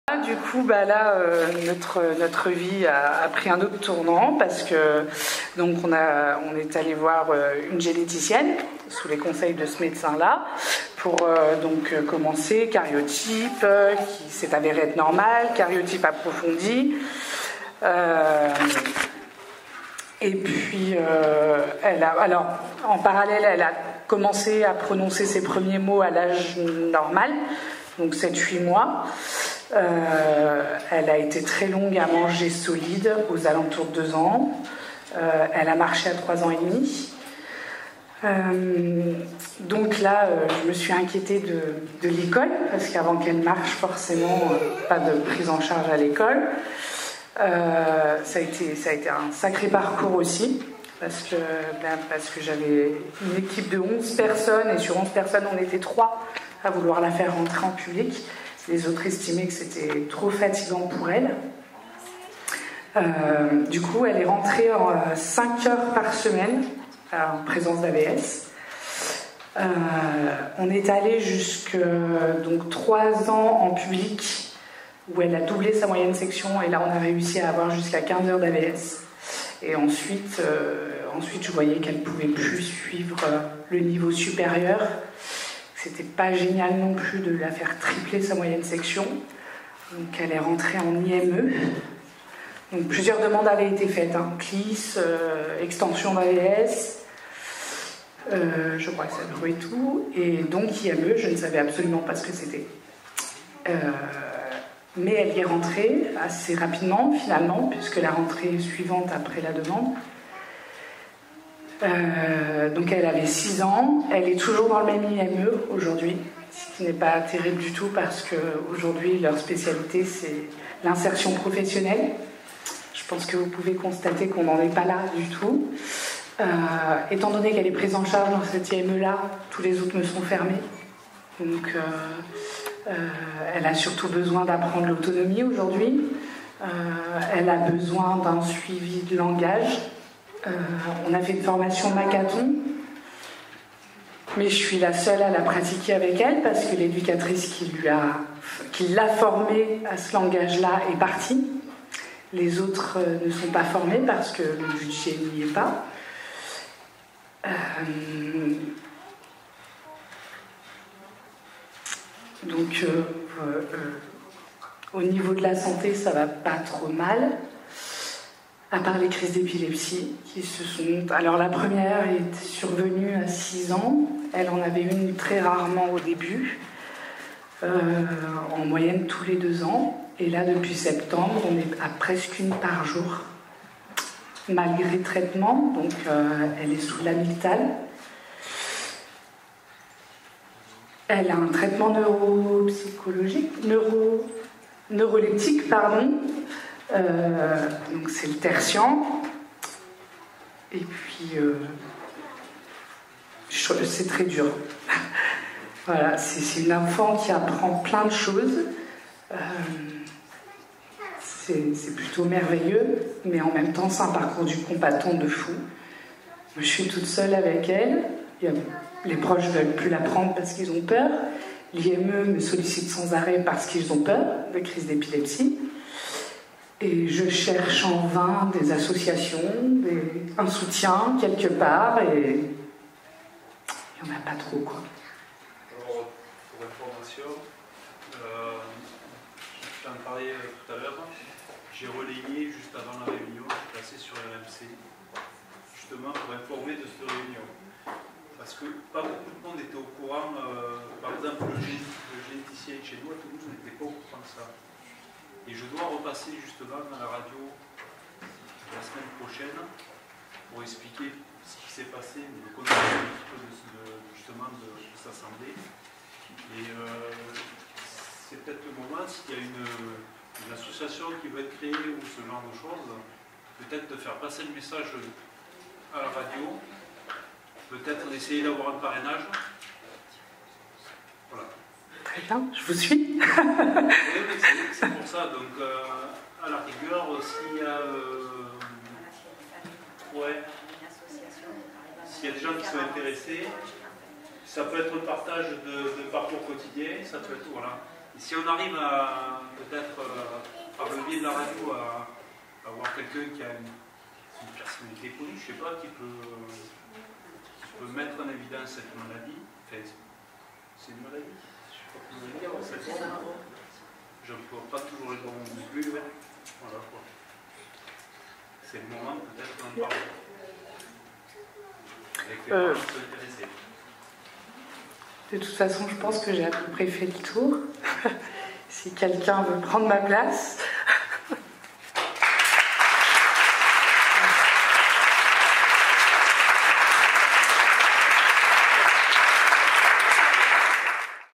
12_Témoignages de familles
Témoignages de familles lors de la journée dédiée aux familles touchées par le syndrome KBG, le 14 juin 2019 au CHU de Rouen.